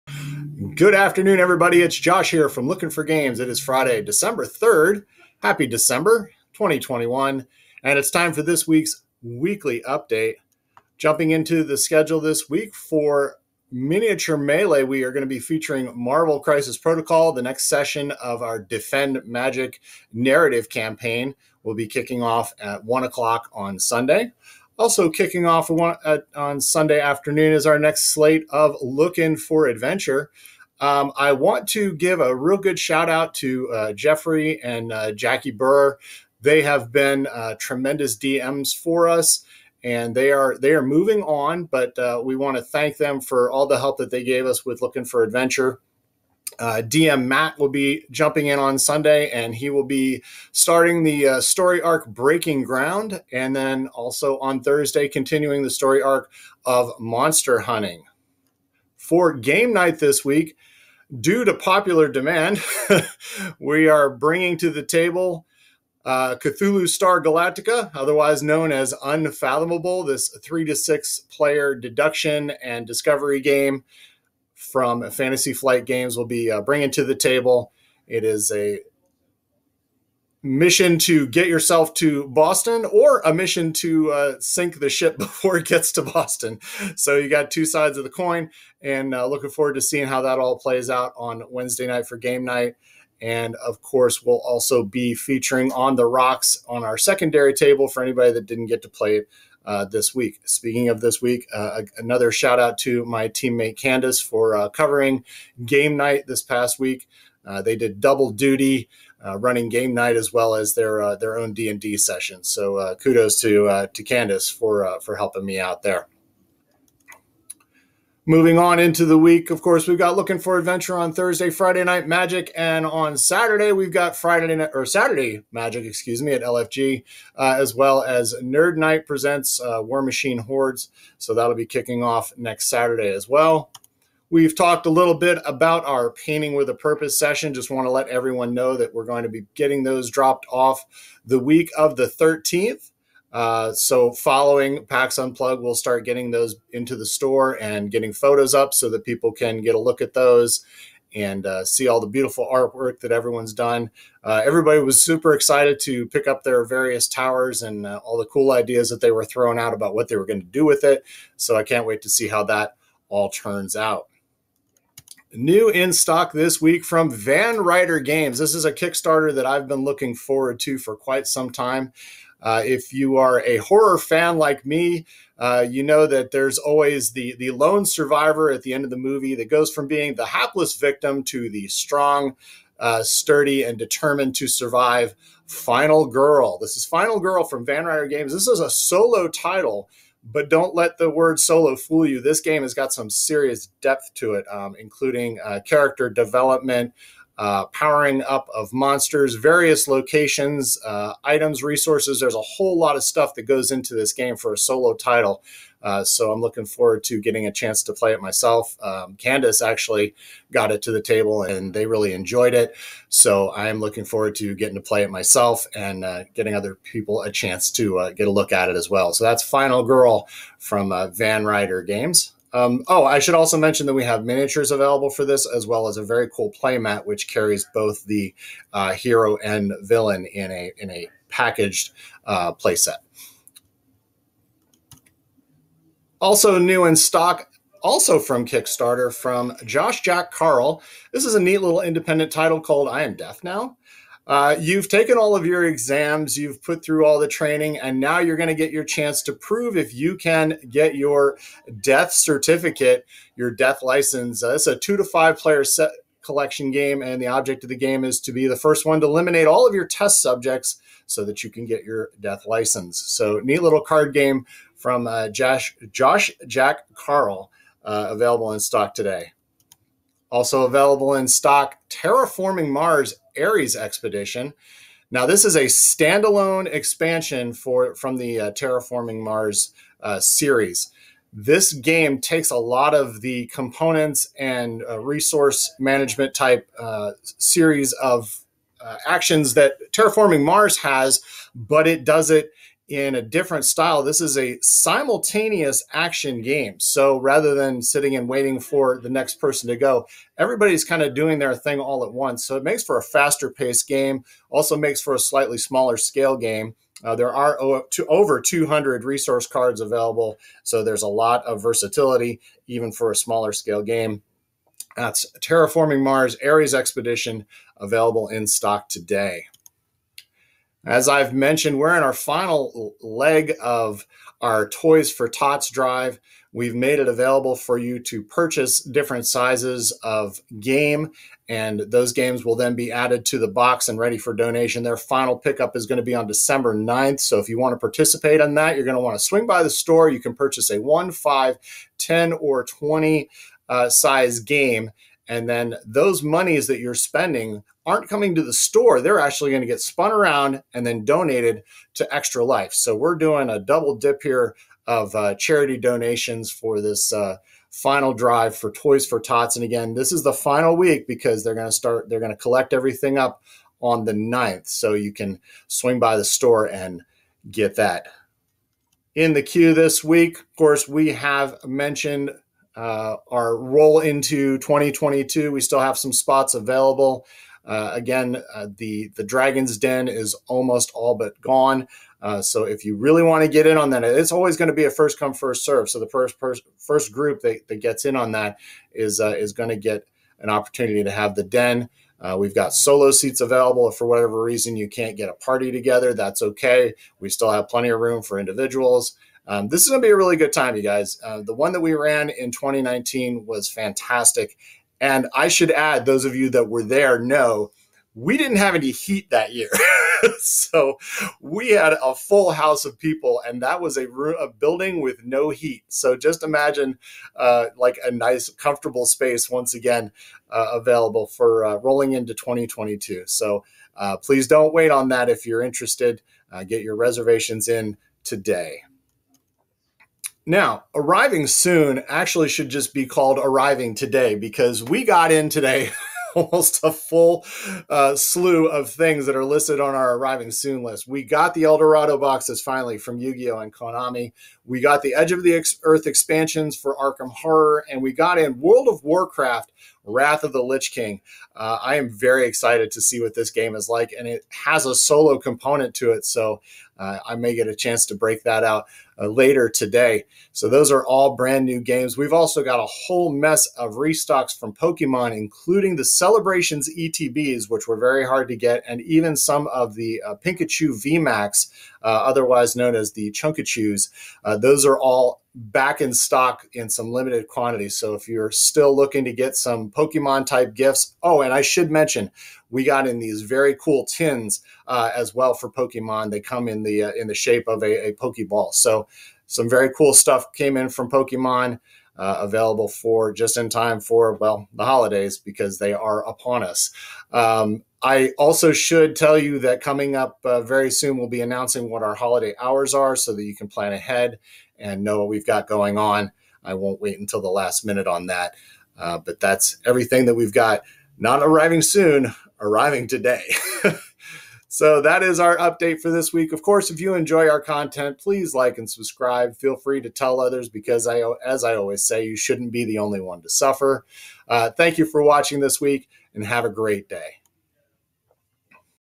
Lookin For Games Weekly Update 12-3-2021
This week we talk about Final Girl from Van Ryder Games, World of Warcraft: Wrath of the Lich King, Pokemon, Yu-Gi-Oh! and a whole mess of surprise stock!